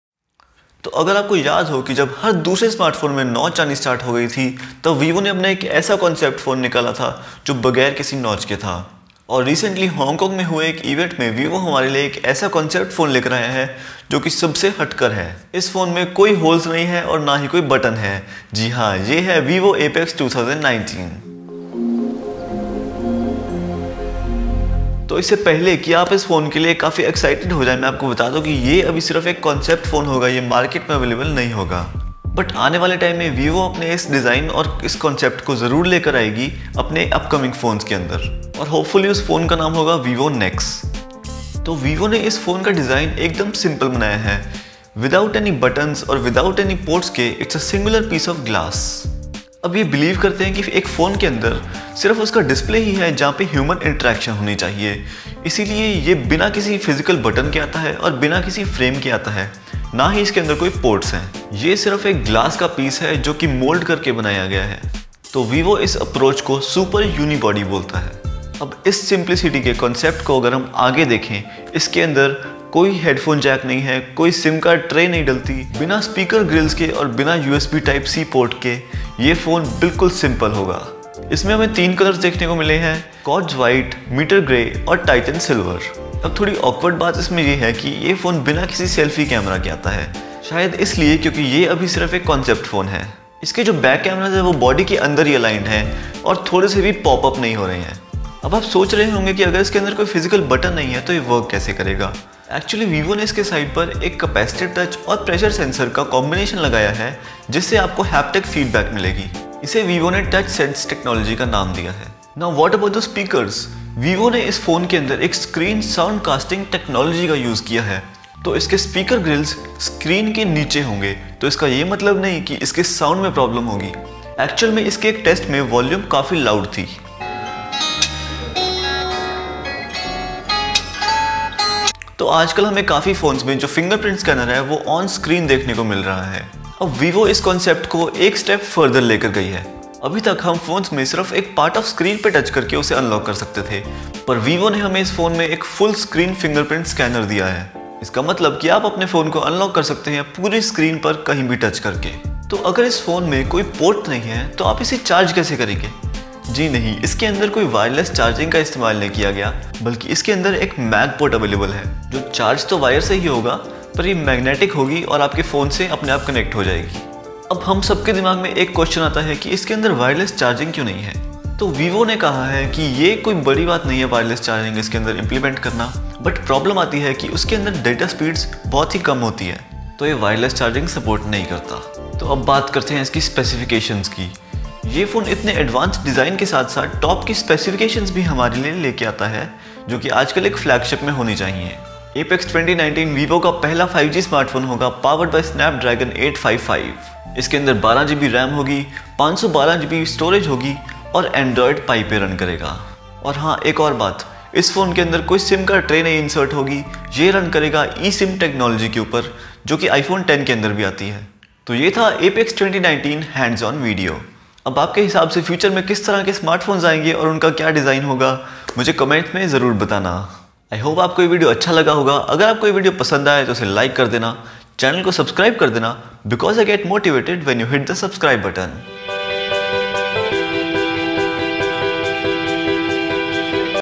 VIVO APEX 2019 | NO BUTTON | NO USB PORT | NO SIM TRAY | FUTURE CONCEPT PHONE | Tech-iela
Vivo Apex 2019 is a new concept phone with no physical buttons no usb port no headphone jack and new future concept. Its a totally bezels less phone. This is a hands on video on this phone.
Also it has full screen fingerprint scanner and e sim support with no sim card tray .